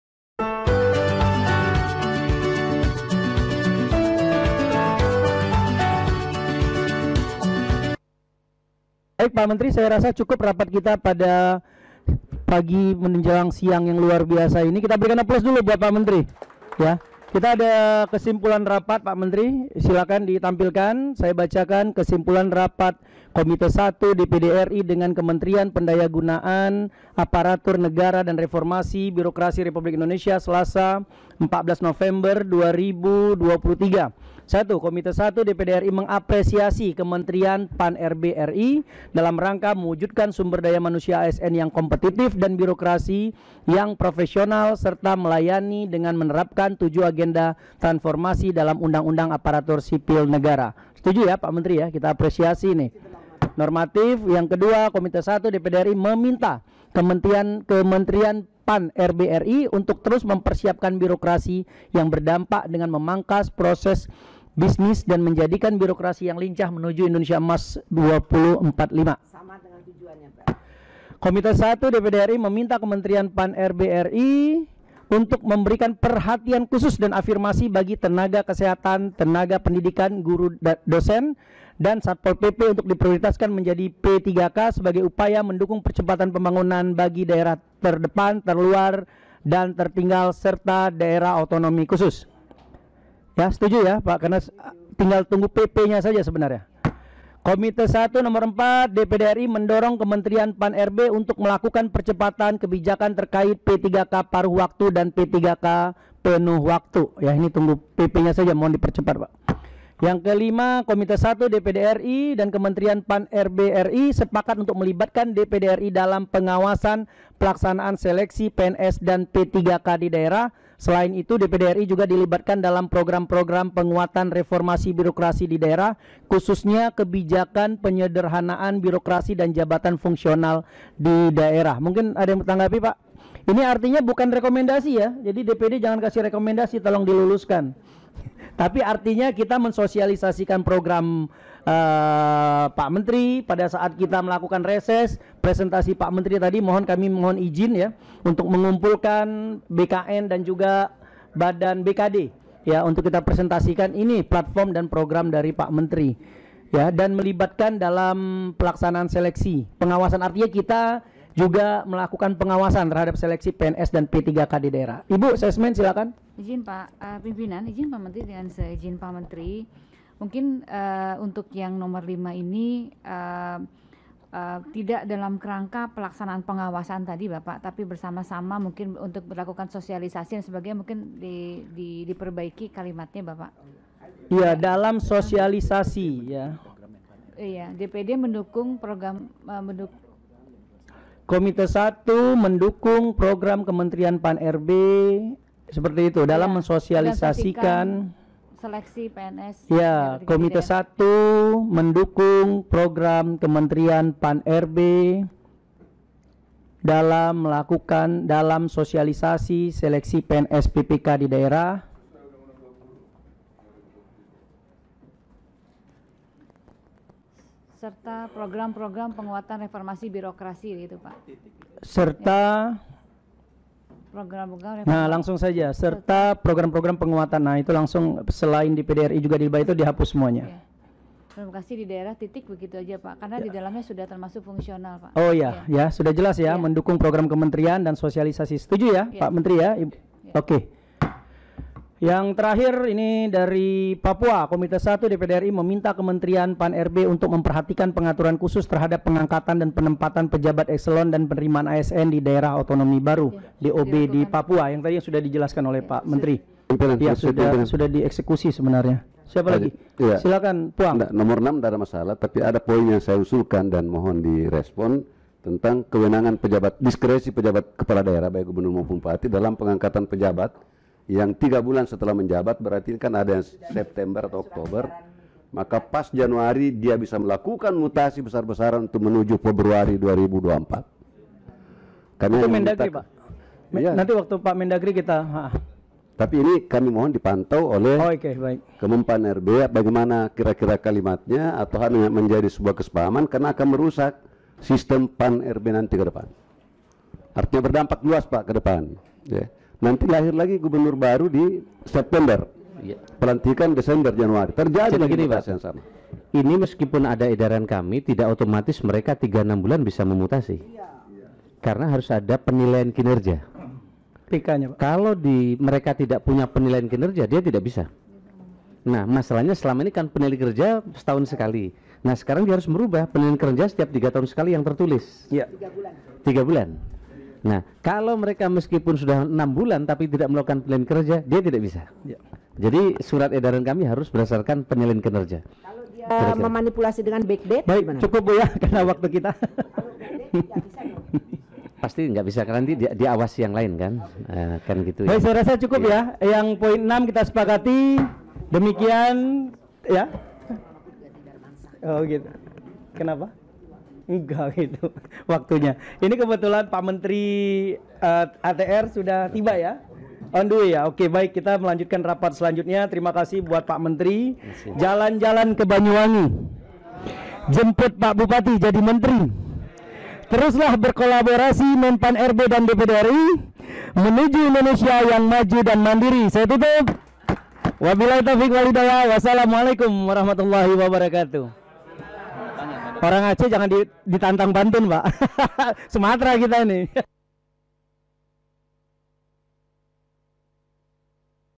Honorer Tenaga Kesehatan, Guru, Dosen & Satpol PP diPrioritaskan Jadi PPPK Tahun 2024
Sumber data/informasi:
Disampaikan dalam Rapat Kerja bersama Komite I DPD RI oleh Menteri Pendayagunaan Aparatur Negara dan Reformasi Birokrasi dalam pembahasan mengenai tenaga Honorer, calon pegawai Negeri Sipil (CPNS), dan seleksi terbuka JPT di Daerah pada tanggal 14 Nopember 2023 di Jakarta